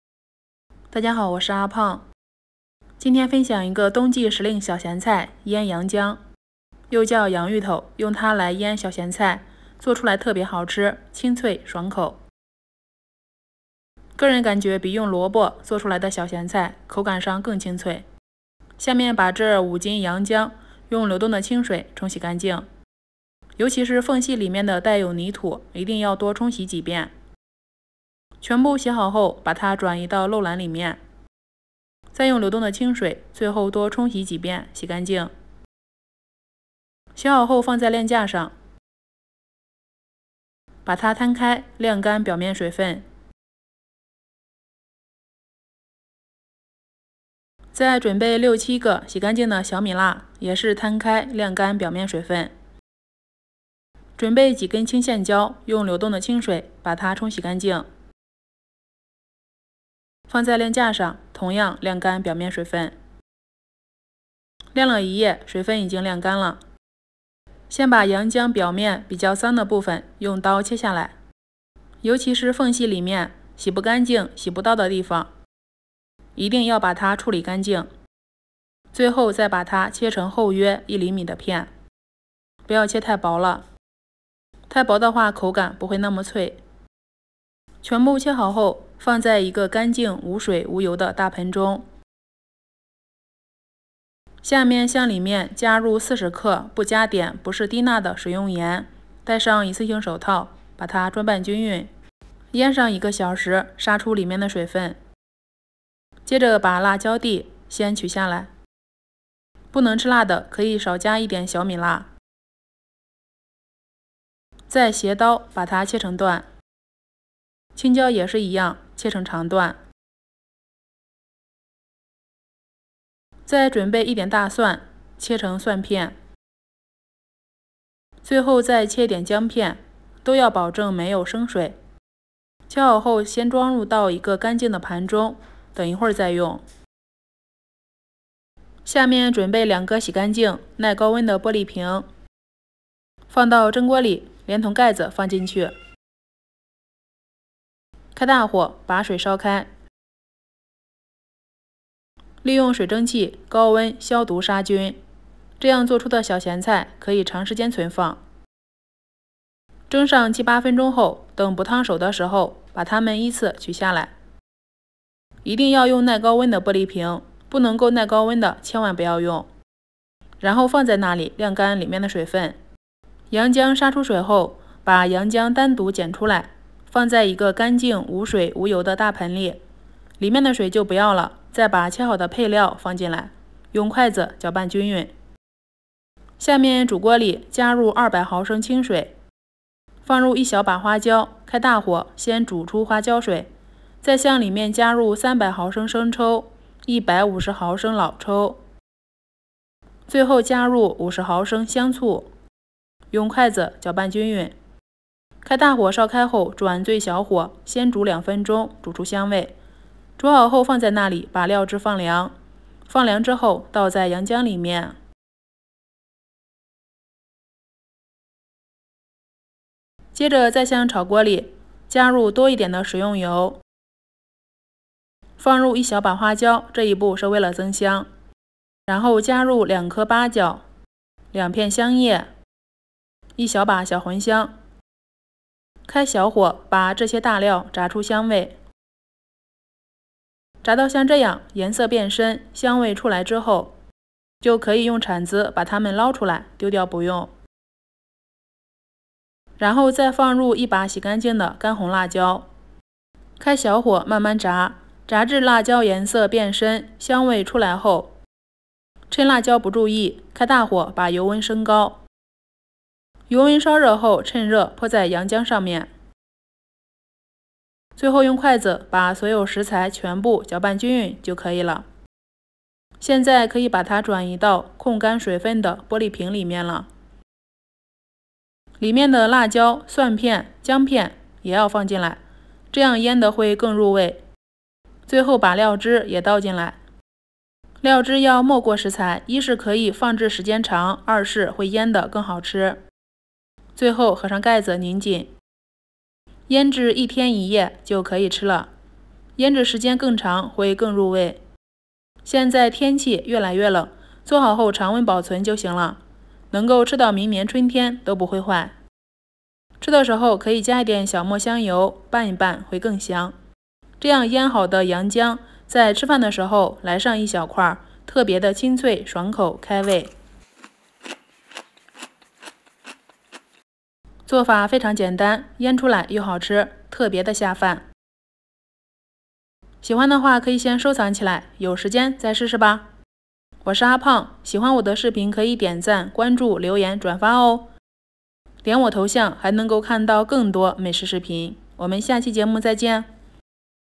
这才是腌洋姜的正确做法，清脆爽口有嚼头，配粥配饭吃一口特别香【阿胖面食】
又到了腌制洋姜的季节，洋姜又叫“鬼子姜”，因其外形跟姜长的很像，而且又是外来植物，所以才被称之为“洋姜”，很多人都喜欢它爽脆的口感和独特的香味，每年的10月到11月份正是洋姜大量上市的时候，人们纷纷买来用各种方法腌制起来，能吃很长时间，喝稀饭、吃火锅、面条的时候夹出一碟，特别开胃解腻！